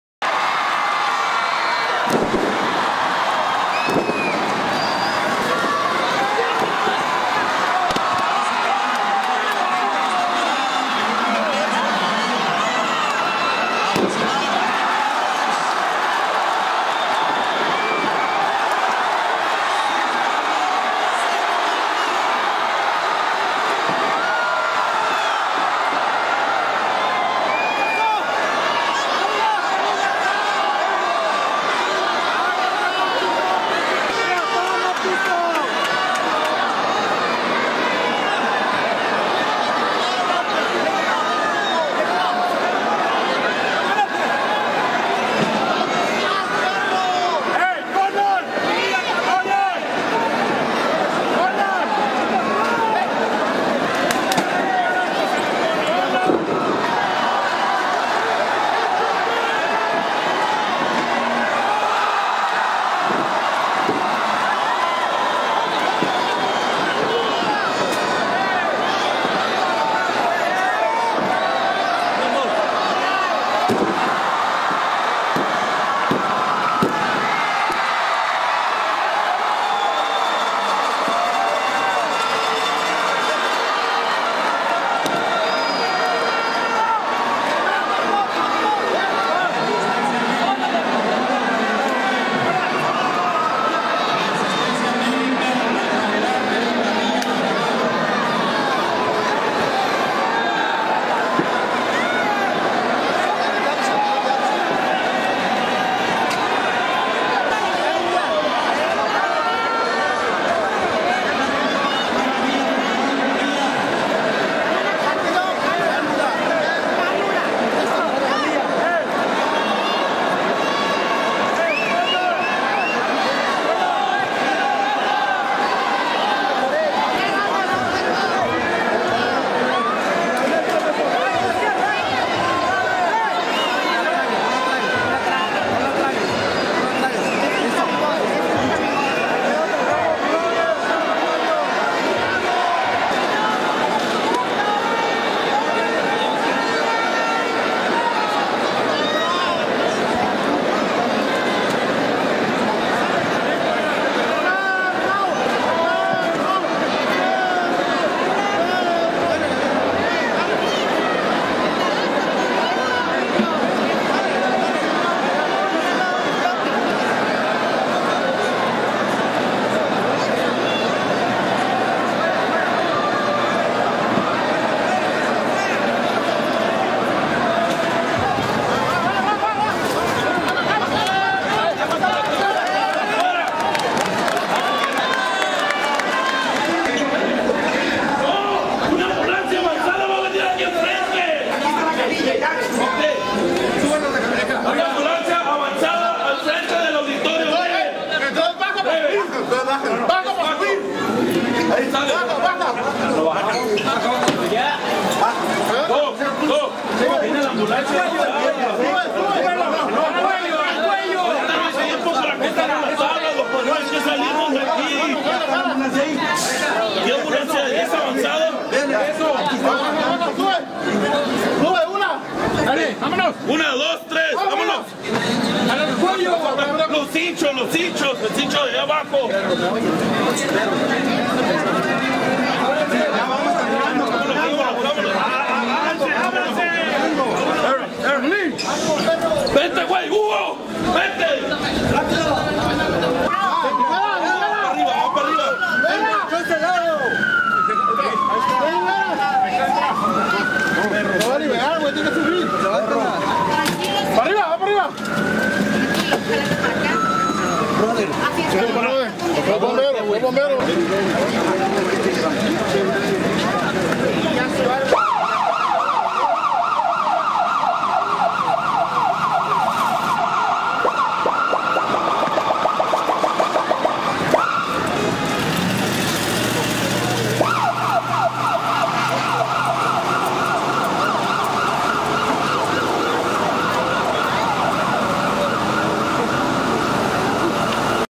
Perro Aguayo Jr Killd in Ring Accident
The Mexican wrestler the son of the dog aguayo passed away in the early hours of Saturday after receiving a blow that it caused a trauma to the head during the function of professional wrestling conducted in the city of tijuana, baja california.